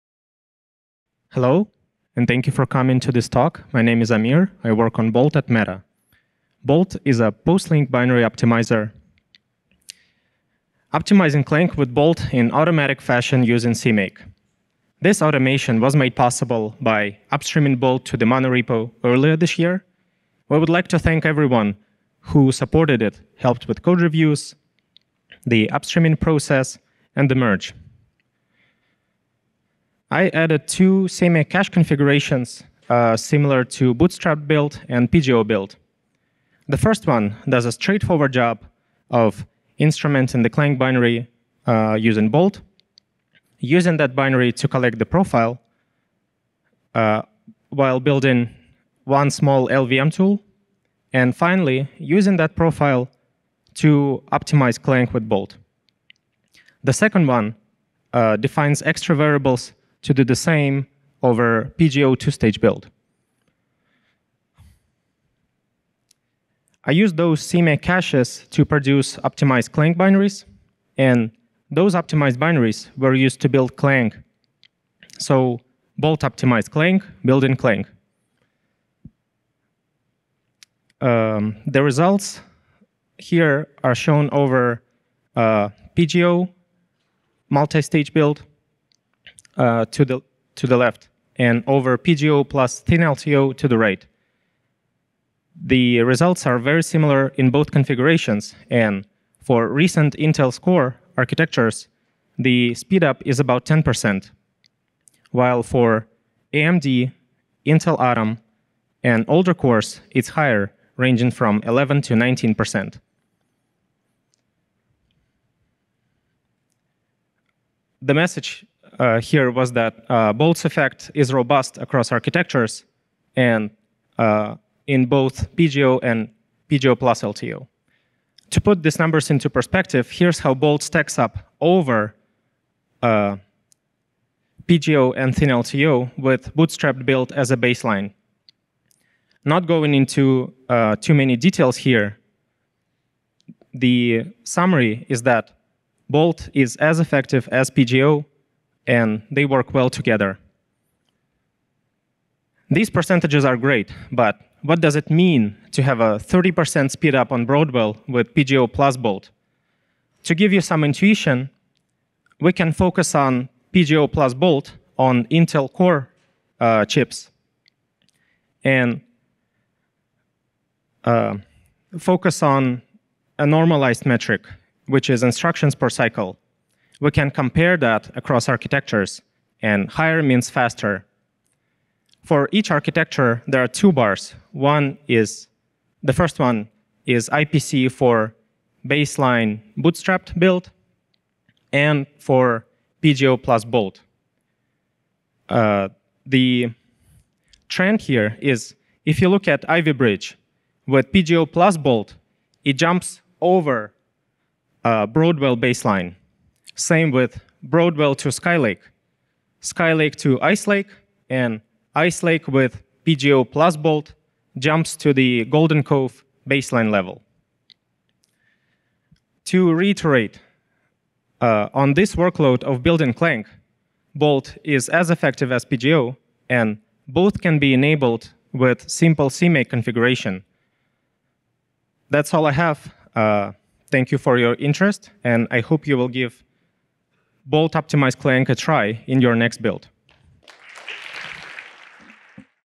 2022 LLVM Dev Mtg: Optimizing Clang with BOLT using CMake
2022 LLVM Developers' Meeting
Optimizing Clang with BOLT using CMake
Speaker: Amir Ayupov
Advanced build configuration with BOLT for faster Clang